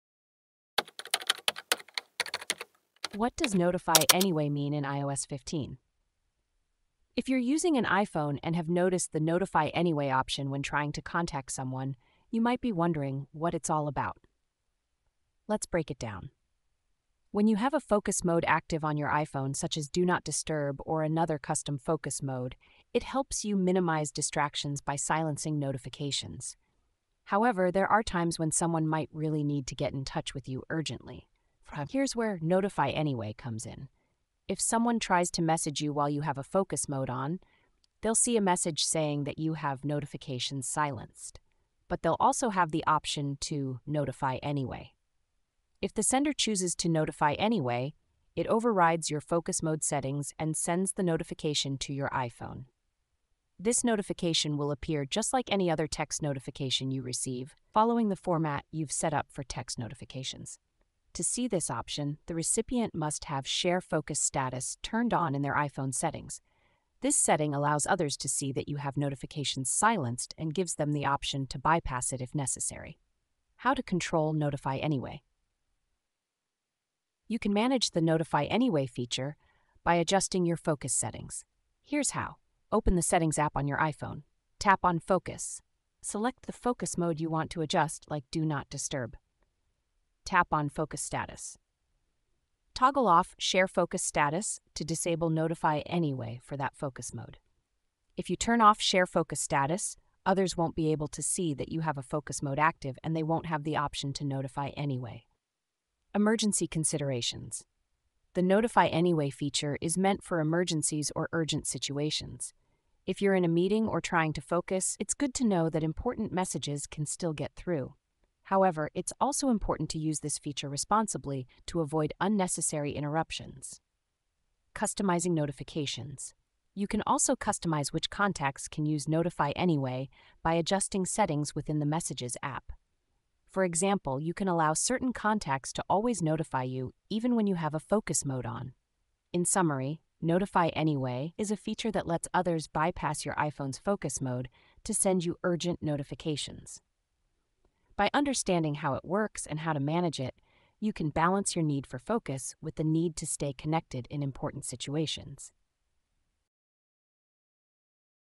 What Does Notify Anyway Mean In iOS 15? - Be App Savvy
What Does Notify Anyway Mean In iOS 15? Have you ever encountered the "Notify Anyway" option on your iPhone and wondered how it works? In this informative video, we’ll break down this feature that allows urgent messages to reach you, even when your Focus mode is activated. We’ll discuss the different Focus modes available on your device and how they help minimize distractions by silencing notifications. You’ll learn how the "Notify Anyway" option appears for those trying to contact you and what it means for both senders and recipients.

We’ll also guide you through the steps to control this feature through your iPhone settings, ensuring that you can manage your notifications effectively. Whether you’re in a meeting or just trying to focus, knowing how to handle important messages can make a difference. Additionally, we’ll touch on customizing your notifications, allowing specific contacts the ability to bypass your Focus mode when necessary.